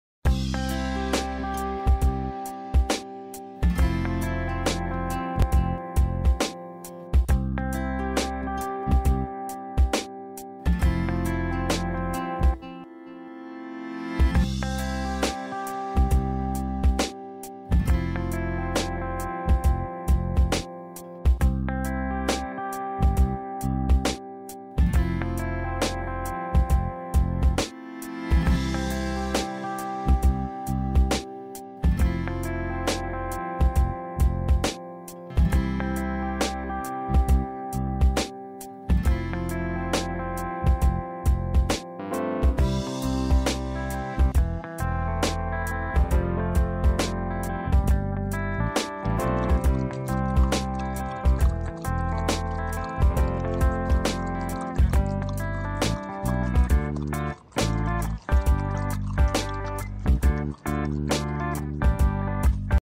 Внимание! Помогите найти садистов! Повесили собачку на леску на дереве. Женщина ее спасла.
Вчера на дереве повесили на леску, маленького пёсика( благо женщина шла к машине  и увидела,сразу подбежала успела спасти. )(Урда, проектный институт ,Пахтакор ориентир)Маленький похож на корги. чистенький,может чей то домашний?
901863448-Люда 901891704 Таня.
Сейчас ищем поблизости камеры,чтобы найти тварей,которые это сделали. Пока его приютили ищем хозяев.